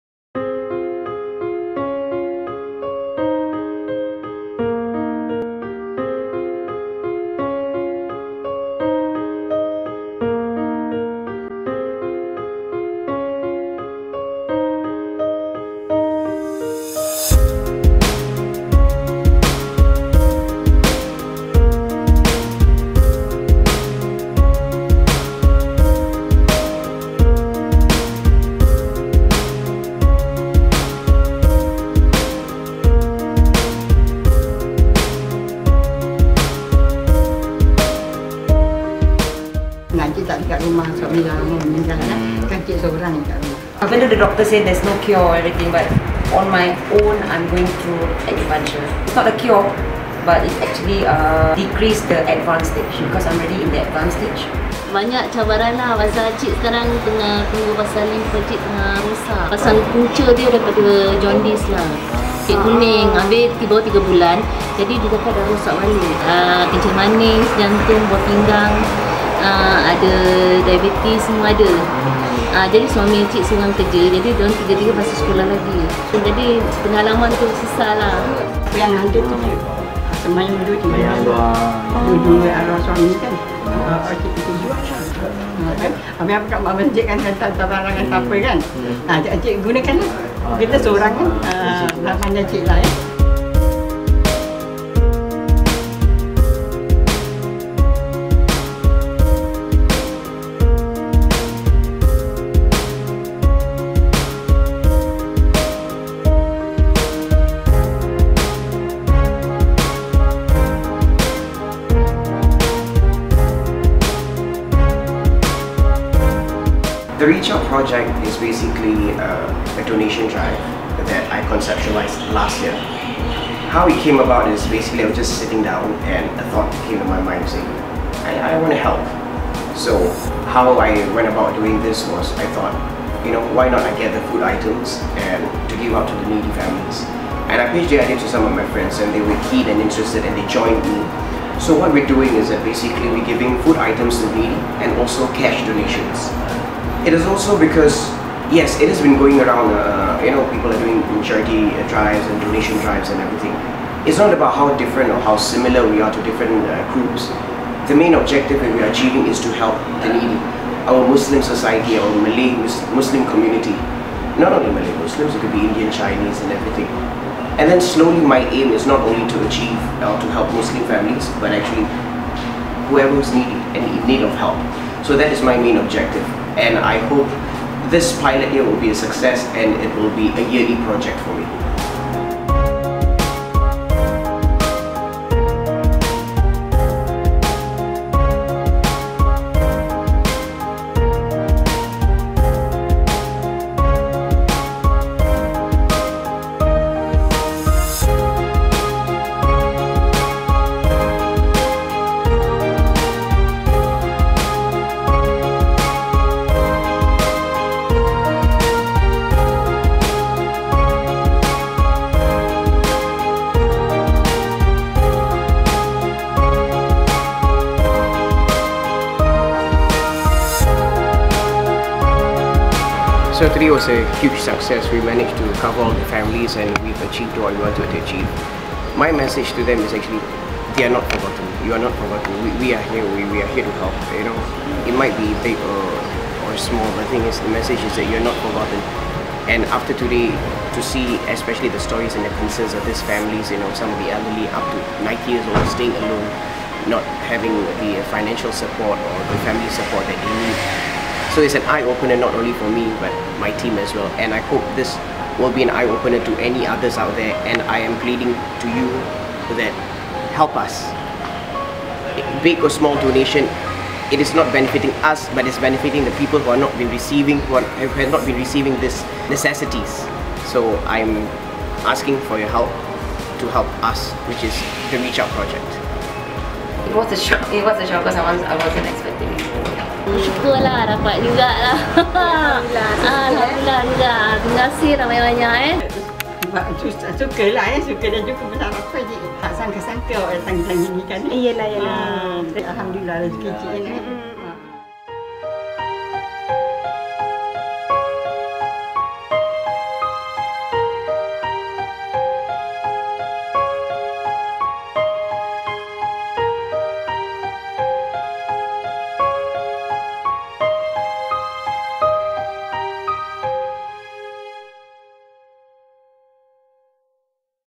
Reach Out Project 2014
Reach Out Project is a charity event initiated by Zamair Ibrahim during the month of Ramadan 2014.